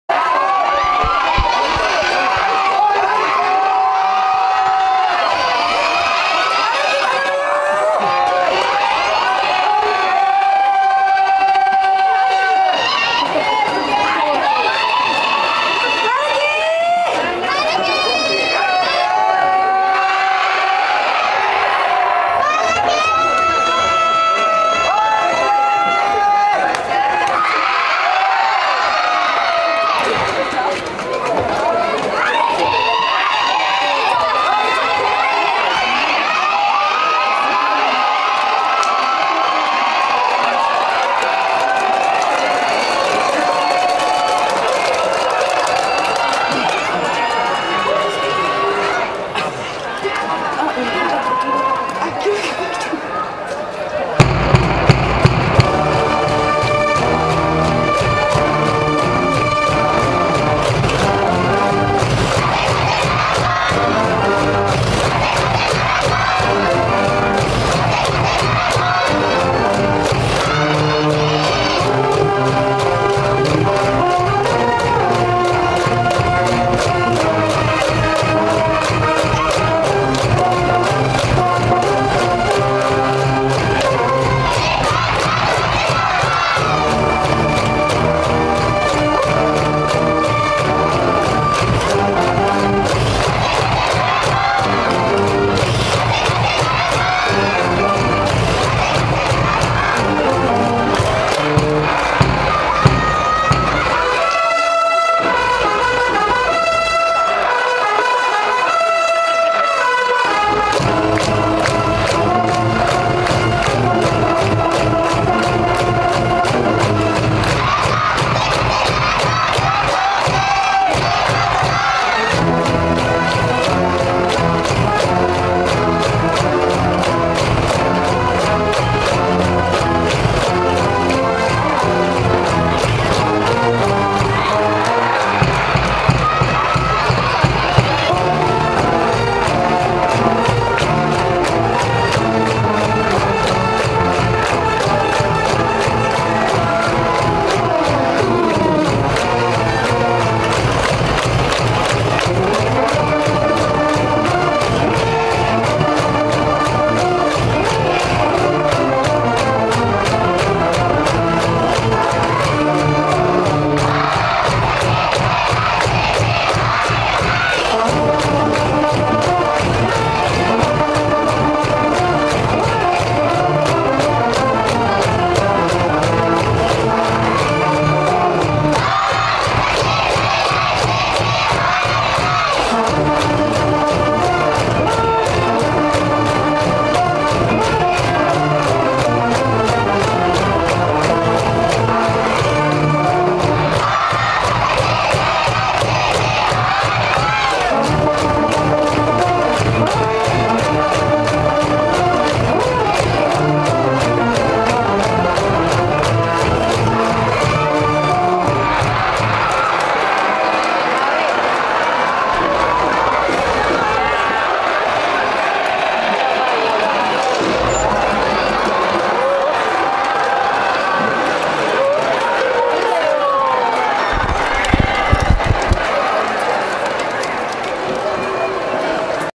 成 田 － 習 志 野 ７回裏
２０１３年７月２４日水曜　第２試合
１３：２０～１６：５６　（３時間３６分）
千葉・ＱＶＣマリンフィールド

第９５回全国高等学校野球選手権千葉大会　準々決勝
成　　　　 田　－　習　 志 　野　（延長１２回サヨナラ）

成　　 田　００１１２００１０　０００　　５１１　１
習 志 野　１１０１００００２　００１　　６１０　１